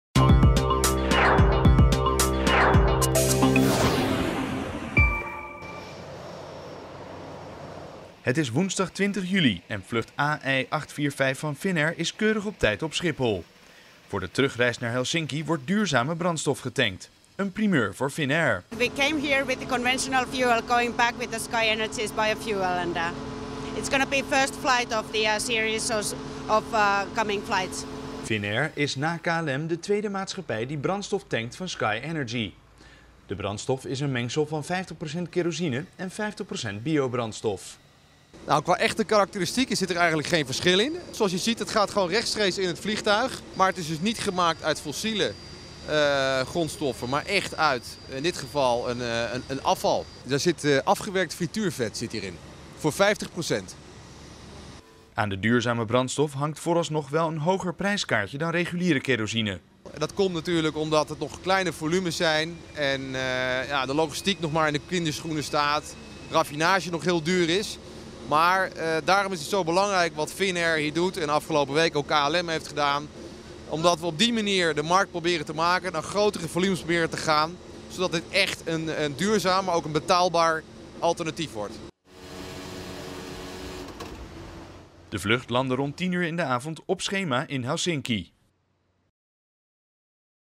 Finnair vliegt duurzaam
Finnair startte op woensdag 20 juli 2011 een duurzame lijndienst van en naar Helsinki. Gisteren werd op Schiphol voor het eerst de biokerosine van SkyNRG getankt.